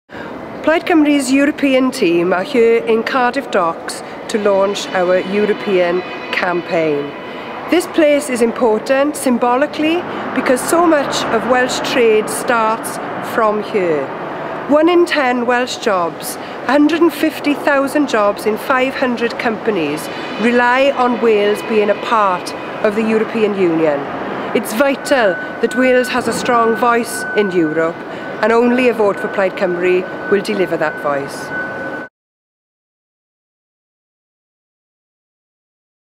Launch of Plaid Cymru European Election Campaign
Leanne Wood AM, Leader of Plaid Cymru, the Party of Wales launches the party's European Election Campaign while on a visit to Cardiff Docks.